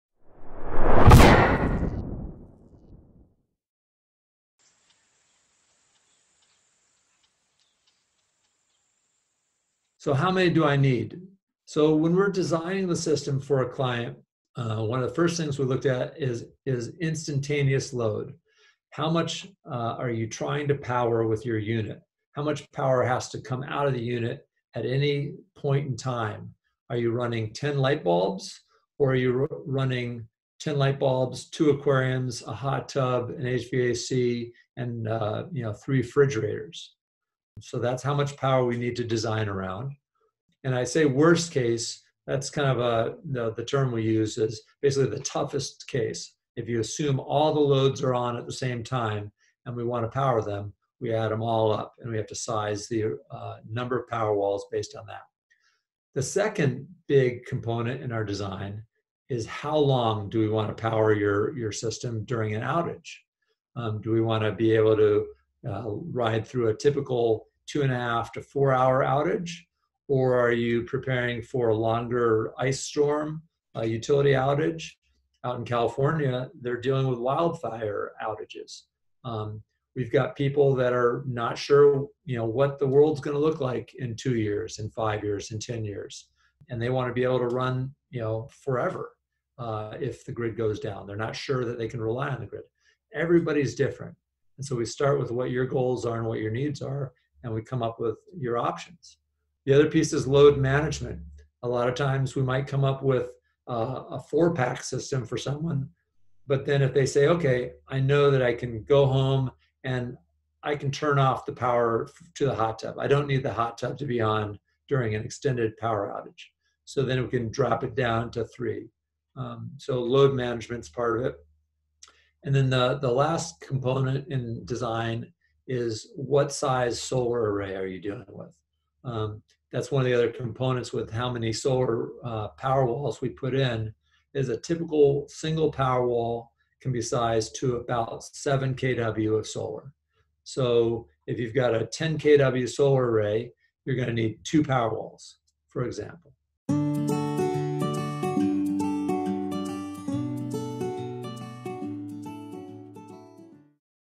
How many Tesla Powerwalls do I need?
Tesla Powerwall  load considerations explained:
1. What do we want to backup?
2. How long do you need backup for?
3. How will you manage your loads during outage?
4. What size solar array do you have?